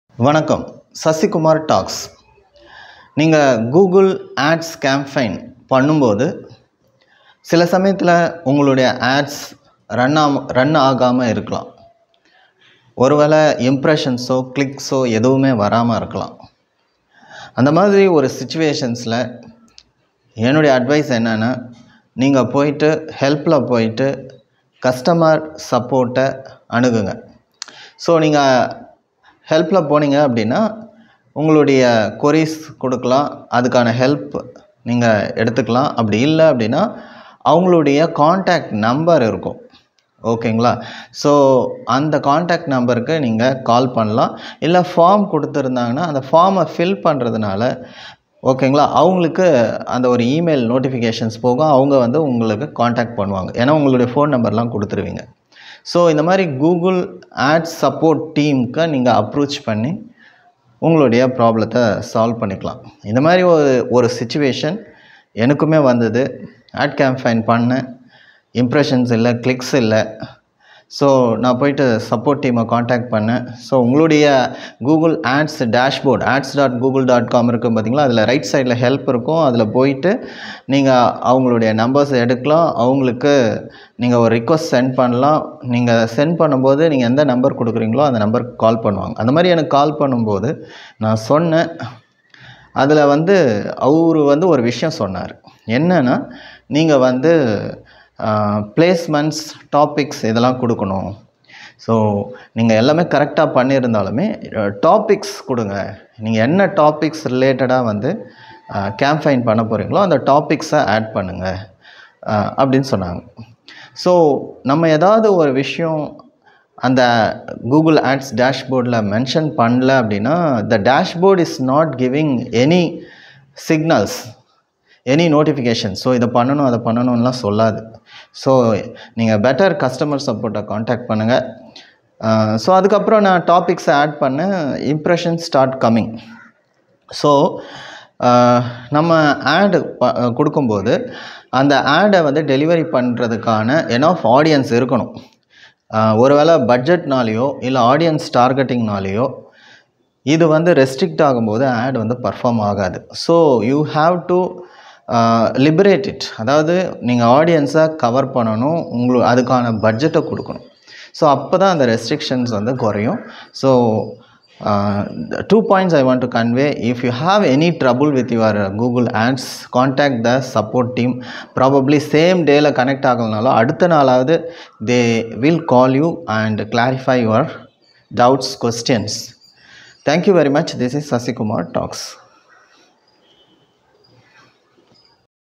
SEO Class 40.8: Google Ads Not Running? Sasikumar Talks (in Tamil)
Hi Friends, Sasikumar Talks here. Today, we're discussing an issue that many face: Google Ads not running.

If your Google Ads aren't running, there are several potential reasons. First, it could be a simple issue like insufficient funds in your account or expired payment information. If that's not the case, it could be due to your ad not being approved by Google's ad policies.

Another reason could be that your bid or your daily budget is too low. Google Ads operates on a bidding system, and if your bid isn't competitive enough, your ads might not show. Similarly, keywords could also play a part. If your keywords aren't relevant or are too broad, your ads might not run.

Finally, technical issues such as incorrect tracking codes or a disapproved website can also cause your ads not to run.

To handle this, first, identify the issue through your Google Ads account. Next, follow Google's guidelines to resolve the problem, whether that's updating payment information, revising your ad, adjusting your bid, refining your keywords, or fixing technical glitches.

Remember, always monitor your campaigns closely to ensure they're running smoothly and bringing you the desired results.

That's it for today's talk on Google Ads not running. Until next time!